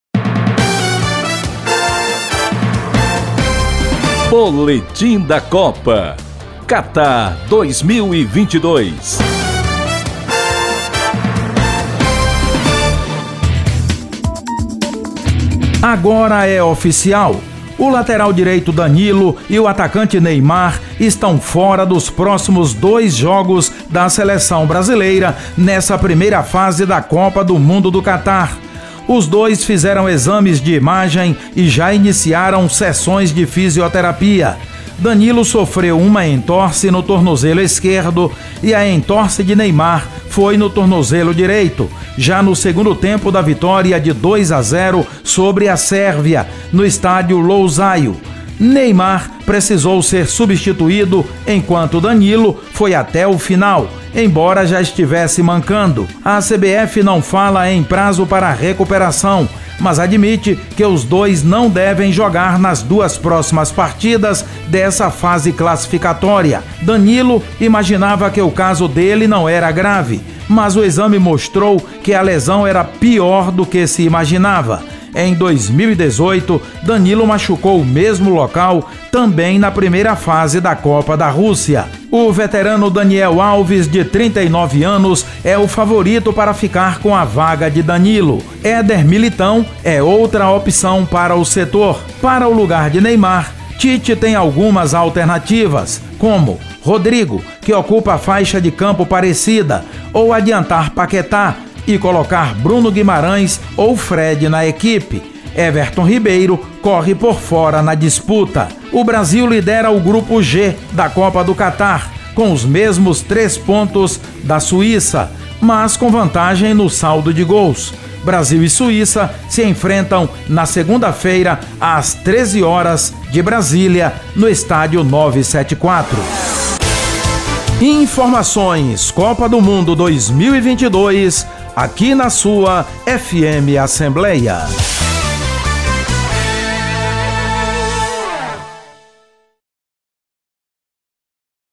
Boletim da Copa #4
A Rádio FM Assembleia apresenta o Boletim da Copa, para a cobertura da competição internacional de futebol, no Qatar. Produzido e apresentado pelo radialista Jota Lacerda, o quadro traz informações sobre a seleção brasileira com detalhes dos adversários do Brasil e avaliações sobre as demais seleções campeãs do mundo.

Produção e narração: Jota Lacerda / Edição de áudio: Jorge Luiz / Sonoplastia: Ronaldo César.

RÁDIO FM ASSEMBLEIA 96,7

Coordenação de programação: Ronaldo César / Gerente geral da Rádio FM Assembleia: Tarciana Campos / Coordenador de comunicação social: Daniel Sampaio